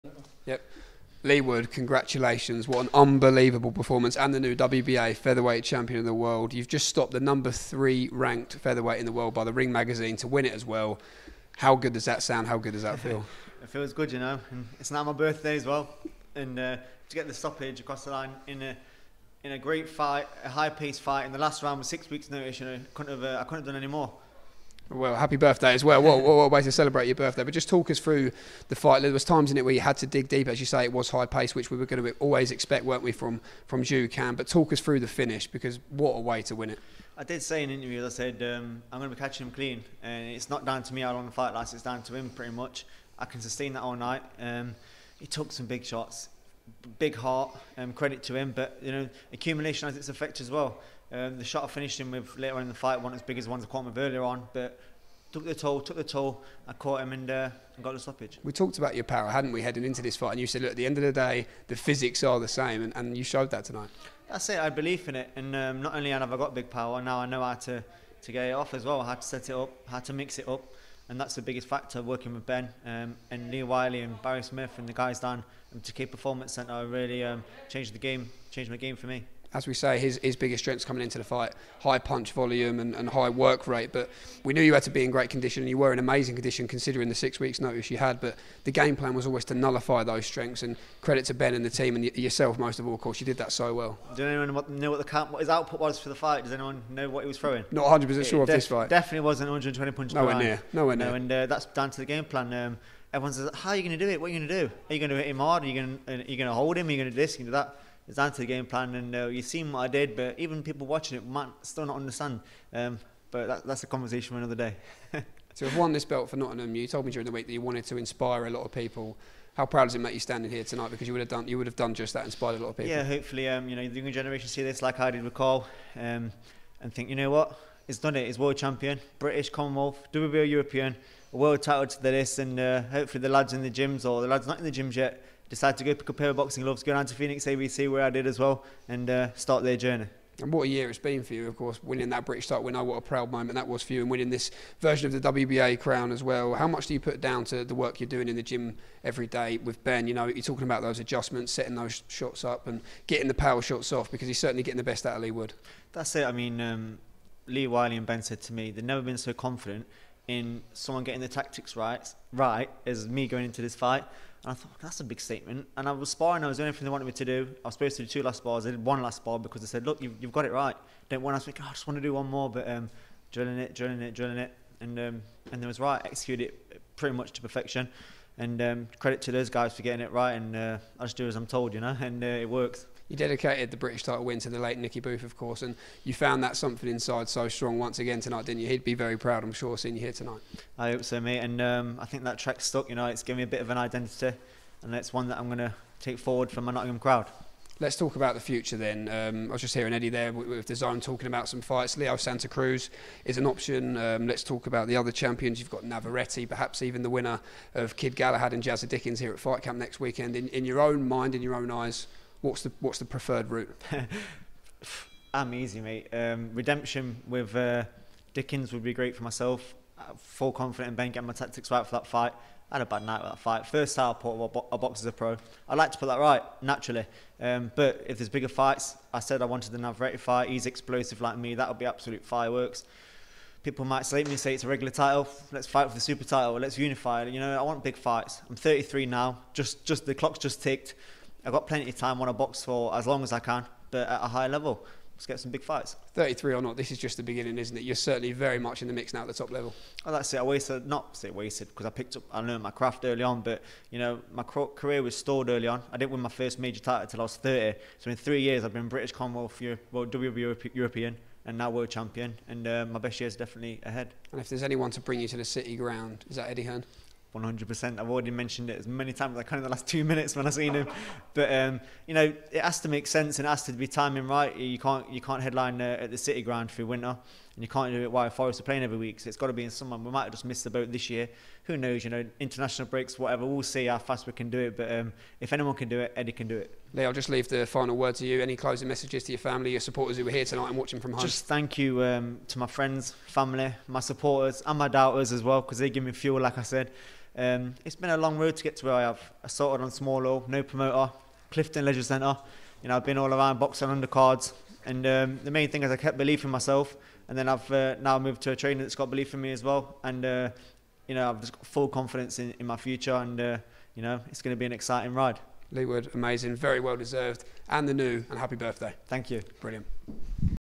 "I want the other Champions!" - Leigh Wood KO's Xu Can to win World Title
WHAT A WIN! Leigh Wood KO's Xu Can in the 12th and final round to win the WBA World Featherweight Title in style at Fight Camp. We catch up with Leigh in the immediate aftermath who makes it clear he wants to jump in with the other belt holders